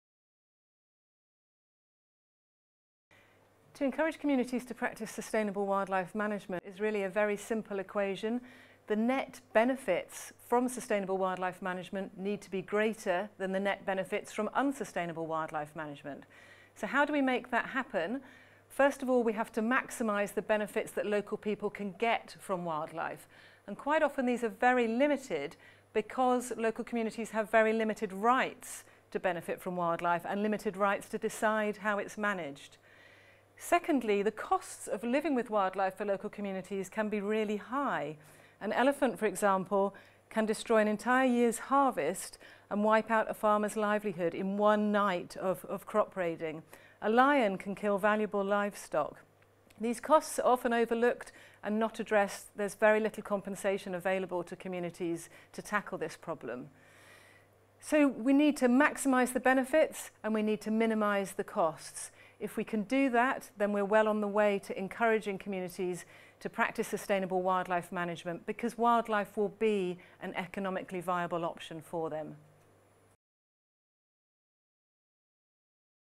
Encouraging communities to practice sustainable wildlife management
Dilys Roe, Team Leader, Biodiversity, International Institute for Environment and Development (IIED), explains that we need to maximize the benefits for local communities of living with wildlife, and minimize the costs, in order to encourage sustainable wildlife management by making it an economically viable option.